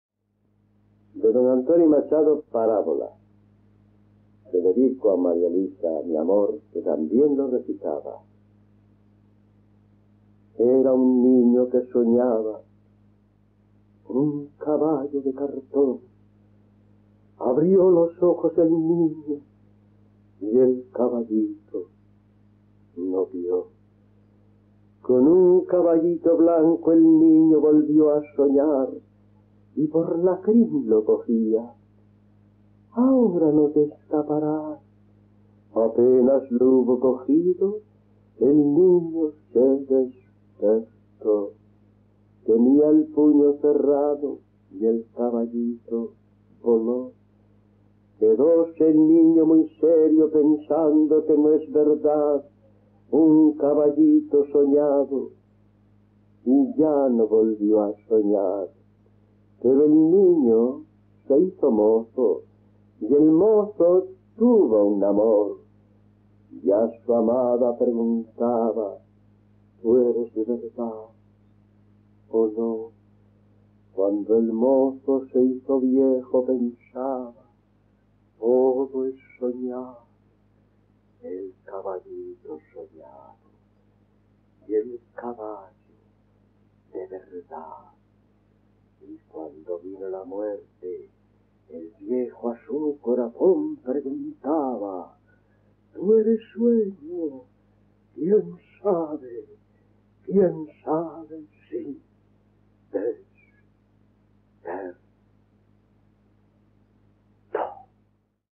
Parabola de Antonio Machado por Pio Muriedas
Pío Fernández Muriedas o Pío Muriedas (también conocido por Pío Fernández Cueto, seudónimo utilizado en el periodo posterior a la guerra civil española, al no poder utilizar el apellido Muriedas). Fue el principal recitador de poesía clásica que ha tenido España en el siglo XX y reconocido por poetas, pintores e intelectuales de dicha época.